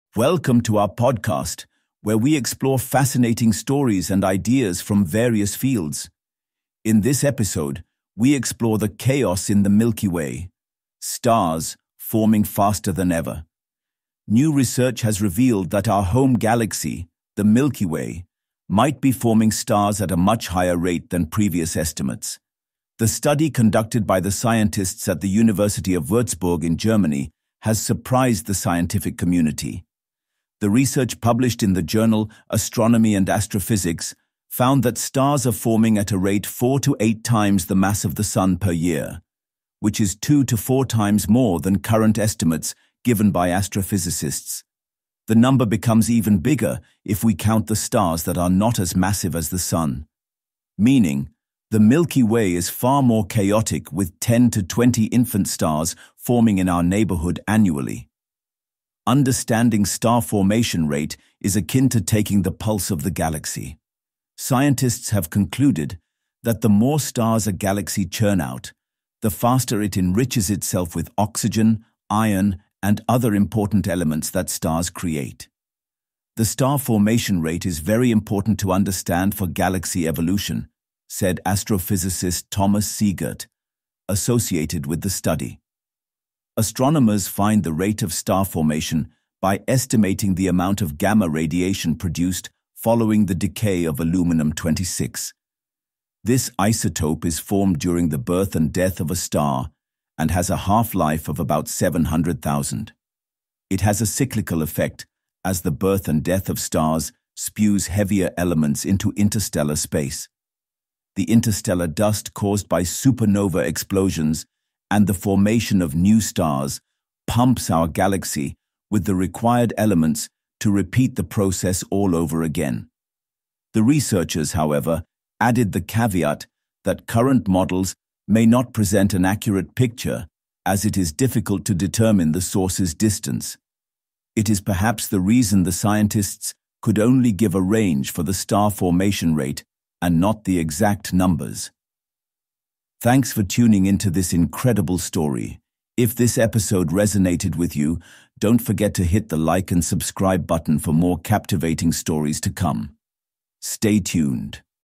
Milky Way’s Star Birth Rate is 4X Higher Than Expected! | WION Podcast
New research reveals that the Milky Way is forming stars at a much faster rate than previously thought. Scientists at the University of Würzburg found that our galaxy produces 10-20 new stars annually, with a mass formation rate 2 to 4 times higher than earlier estimates. The study, published in Astronomy & Astrophysics, suggests a more chaotic and dynamic star-forming environment. Researchers used gamma radiation from aluminum-26 decay to measure this activity. This discovery reshapes our understanding of galaxy evolution and element formation in space. However, scientists note that current models may still be incomplete.